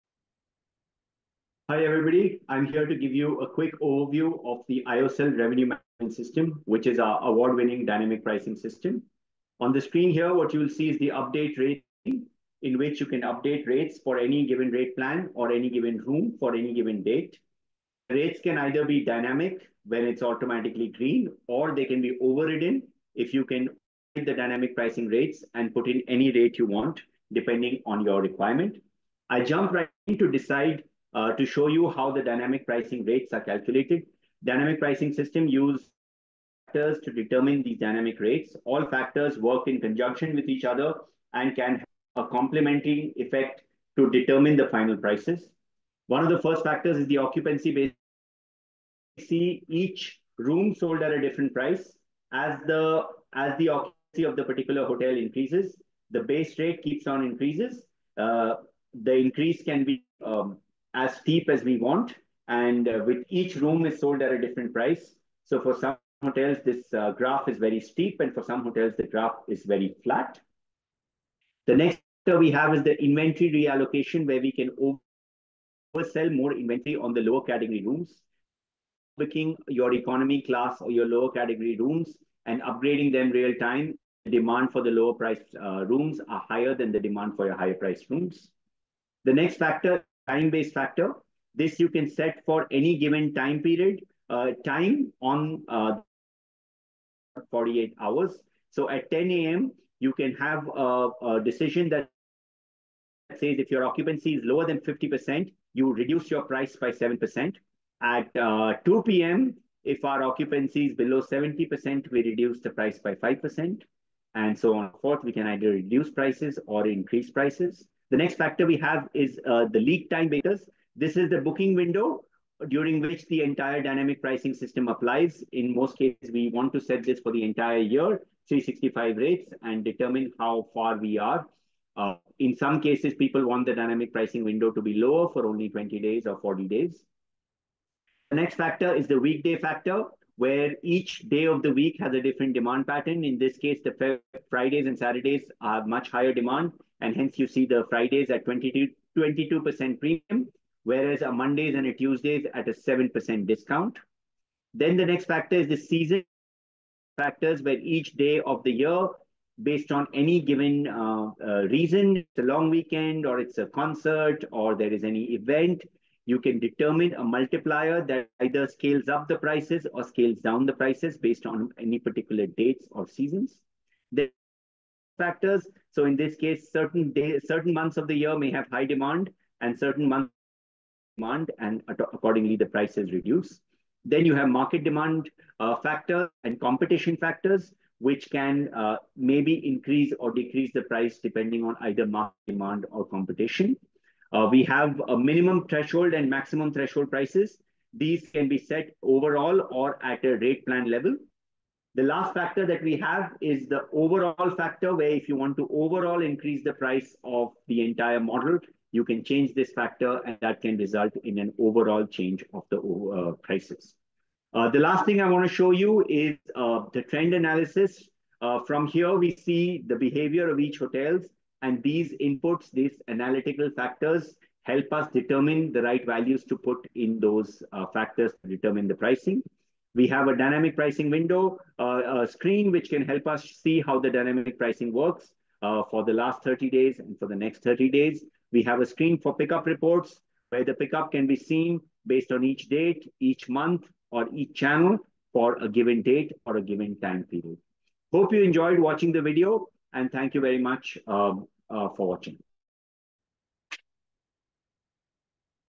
Aiosell Product Demo - Dynamic Pricing System
Fully-automated Revenue Management & Dynamic Pricing System  which uses AI to help you set & maintain optimal room rates in order to make strategic pricing decisions that drive maximum profits.

00:00 Intro
00:10 Update Rates
00:33 Dynamic Pricing Factors
03:30 Analytics - Trend Analysis
03:50 Analytics - Dynamic Pricing Graph
03:58 Analytics - Pickup Report
04:05 Outro